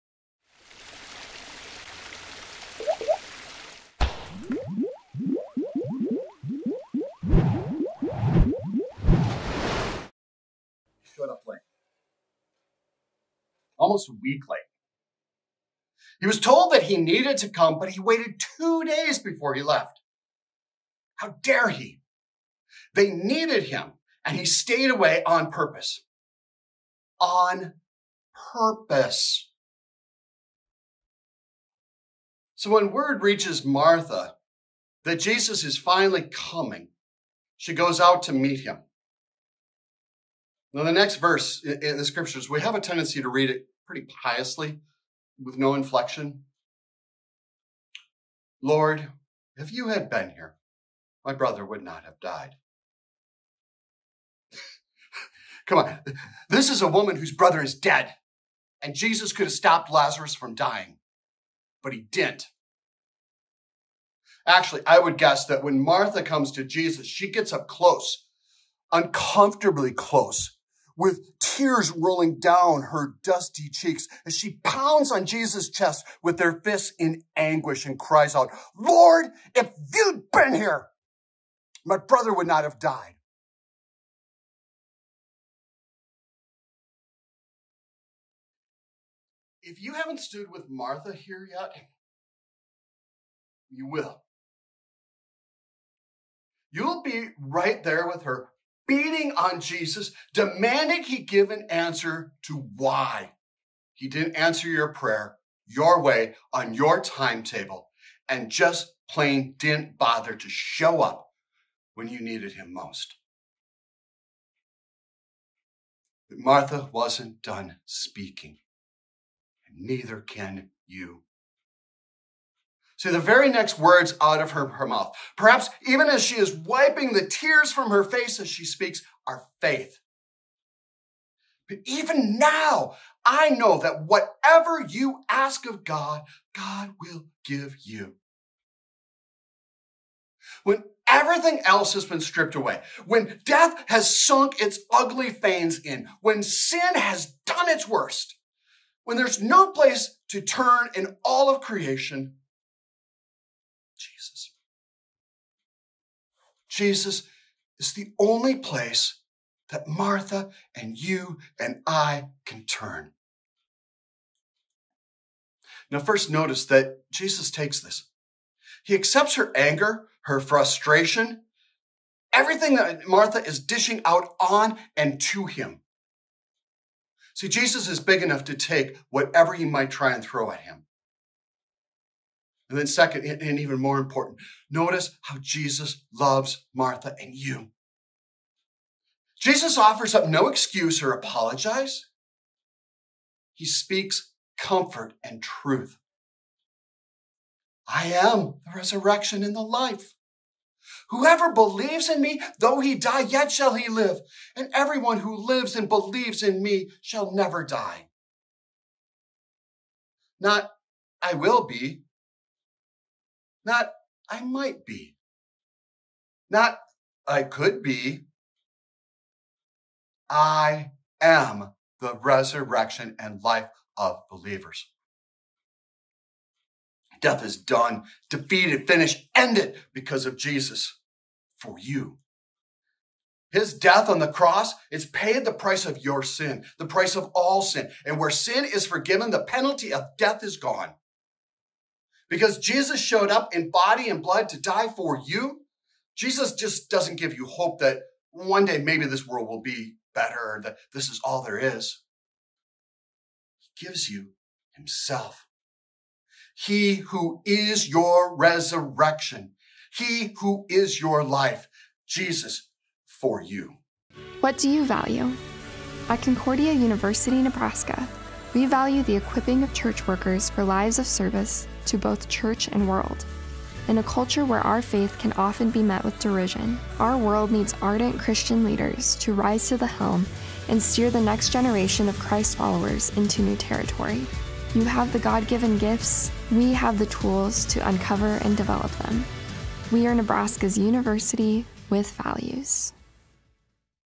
He is the Resurrection and the Life
Pastor Voth talks about Jesus being your resurrection and your life.

Host: Pastor Daniel Voth is a Pastor of Immanuel Lutheran Church in Grand Forks, ND.